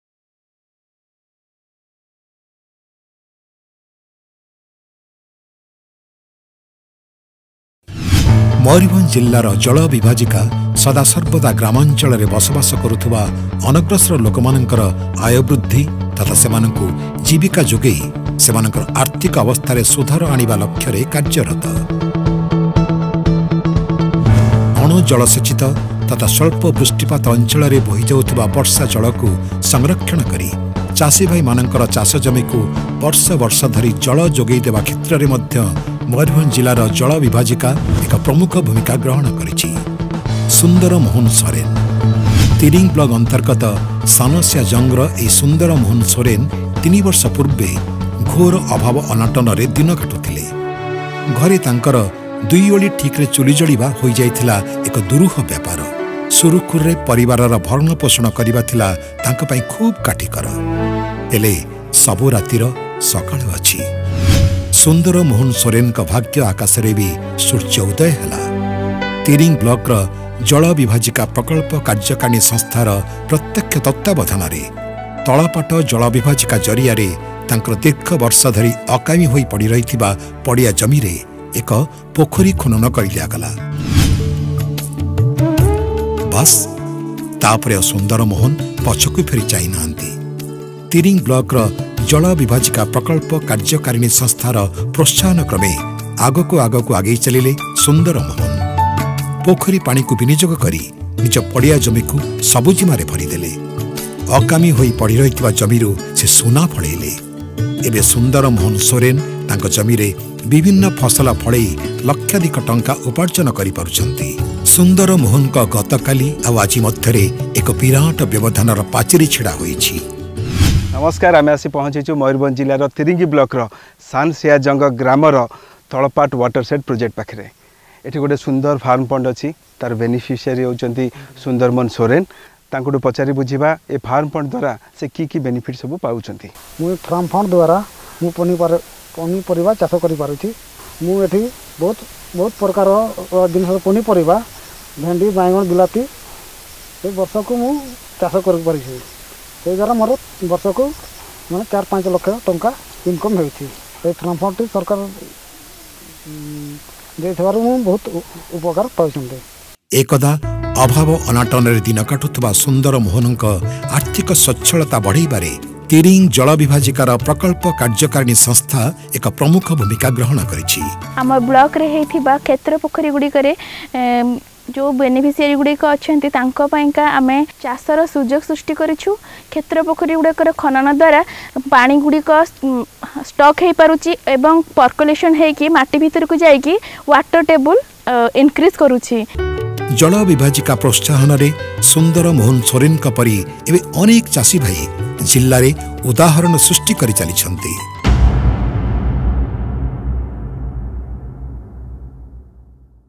ସୁନ୍ଦରମୋହନ କାଲି ଓ ଆଜି
ମୟୁରଭଞ୍ଜ ଜିଲ୍ଲାର ଅଗ୍ରଣୀ_ଚାଷୀ ସୁନ୍ଦର ମୋହନ ସୋରେନ ଅଣ ଜଳସେଚିତ ତଥା ସ୍ୱଳ୍ପ ବୃଷ୍ଟିପାତ ଅଂଚଳରେ ବର୍ଷା ଜଳକୁ ସଂରକ୍ଷଣ କରି ପୋଖରୀ ଖନନ କରି ବିଭିନ୍ନ ଫସଲ ଫଳାଇ ଲକ୍ଷାଧିକ ଟଙ୍କା ଉପାର୍ଜନ କରିପାରୁଛନ୍ତି